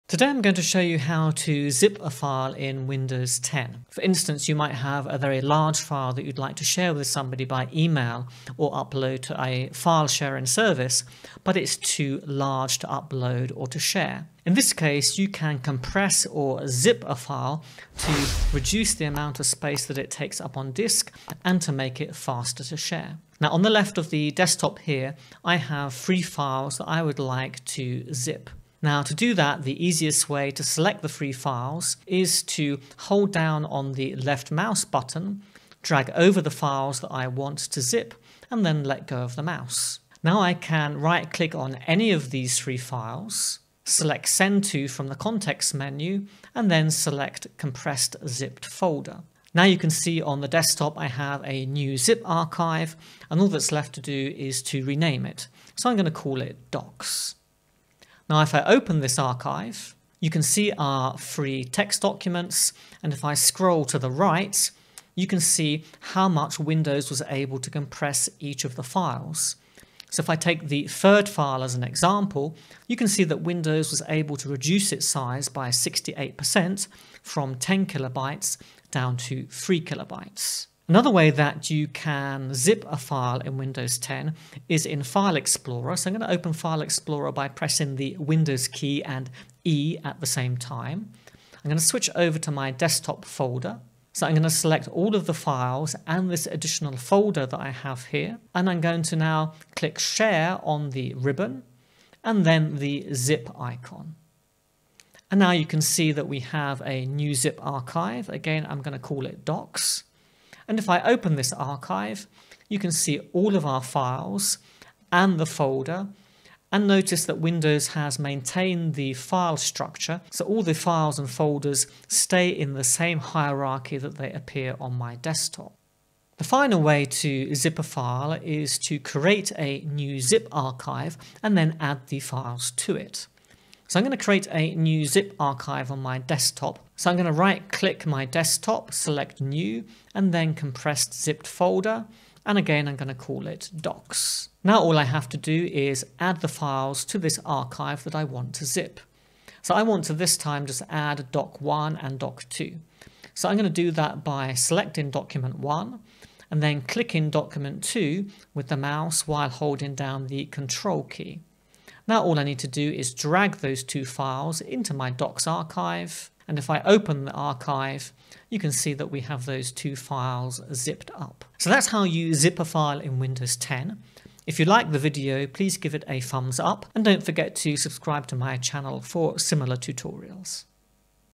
📁 How to Zip a File in Windows 10
In this short tutorial, I'll show you how to zip a file in Windows 10.

Using the built-in tools, I'll show you how to make zip file in Windows 10 and create a compressed archive that is easy to share. Making a zip file reduces its size on disk and makes it faster to upload or send by email. You can also create a zip archive to combine several files into one.

Anyone can follow this tutorial, regardless of whether you are using a desktop PC, notebook, or other type of device running Windows 10. You can follow the same process for Windows 7, Windows 8, and Windows 8.1.

How to make zip file?
Right click the file, and select Send to from the context menu. Click Compress (zipped) folder and then give the new zip archive a name.

What is a zipped file?
A zipped files is a compressed archive or one or more files that you can use to reduce the space your files take up on disk. Zipped files are also useful when you want to share large files or share many files at the same time.
Windows 10 supports the .zip format, which is the most common zip archive type in Windows.

⌚ Timestamps
00:00 What are ZIP files?
00:23 Zip files using 'Send to'
01:20 Zip files using the Share tab in File Explorer
02:06 Create an empty zip archive and add files/folders to it